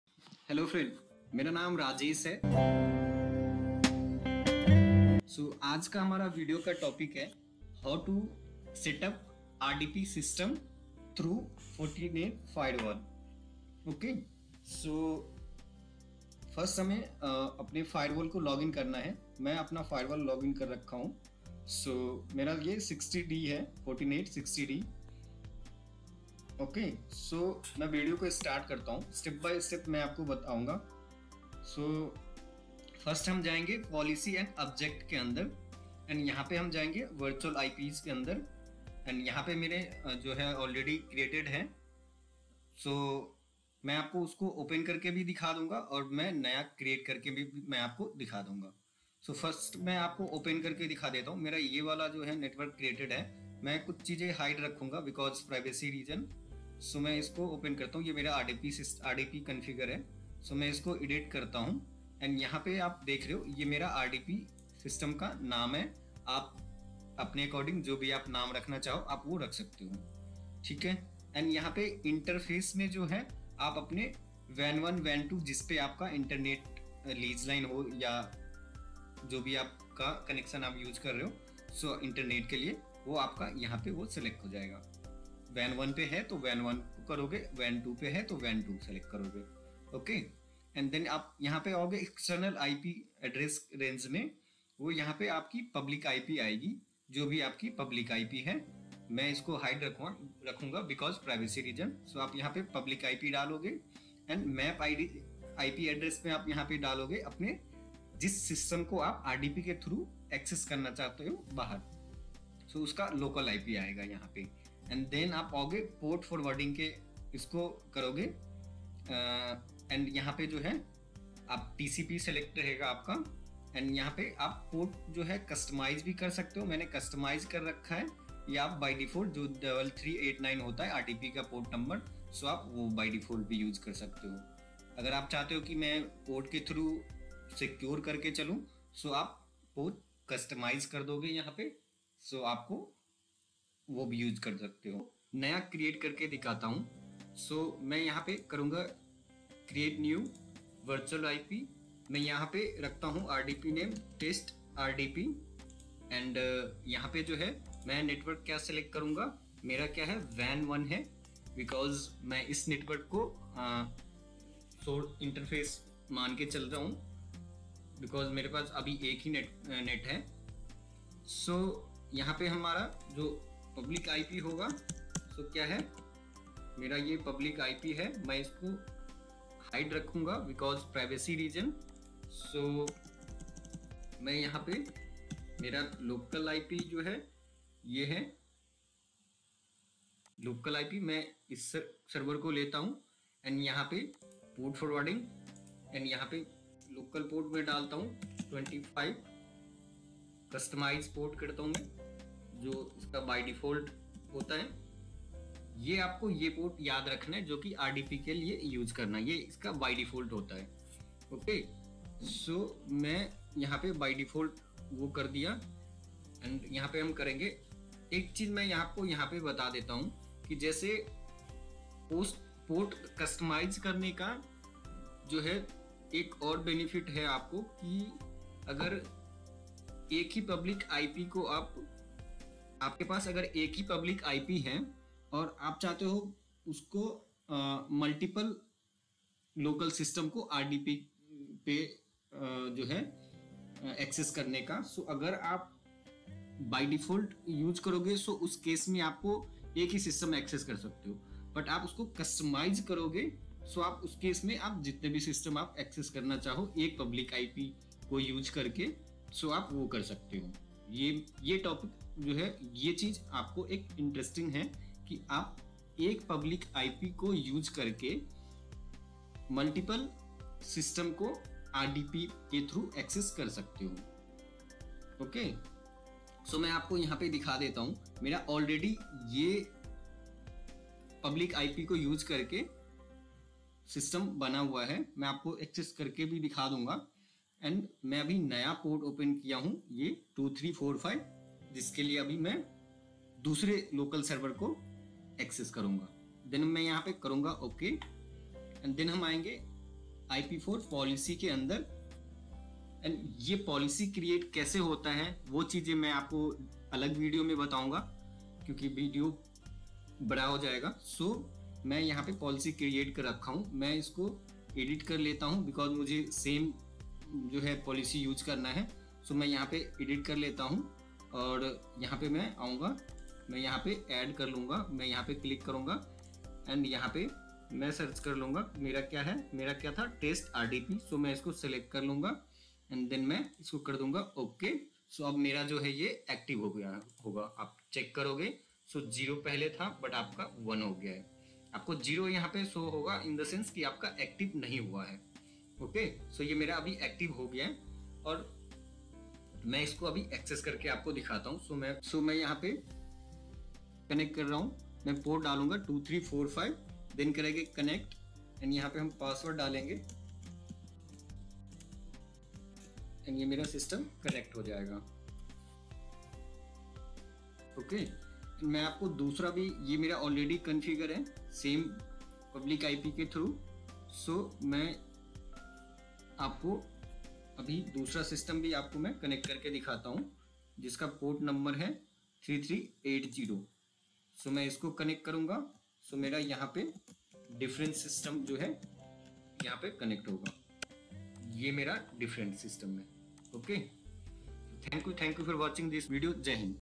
Unlocking Remote Desktop :Trending Tips for Setting Up RDP in Fortinet Firewall (Hindi Subtitle)
In this comprehensive Fortinet Firewall RDP Configuration Guide, you'll discover a step-by-step tutorial for setting up Remote Desktop Protocol (RDP) access securely through your Fortinet Firewall. Stay ahead in 2024 with this up-to-date guide, designed to simplify the configuration process and ensure seamless remote access to your network resources. From initial setup to advanced configurations, this tutorial covers all you need to know to enhance your network's accessibility while maintaining robust security measures. Whether you're a novice or an experienced IT professional, this guide provides clear instructions and insights to streamline your RDP setup within the Fortinet Firewall environment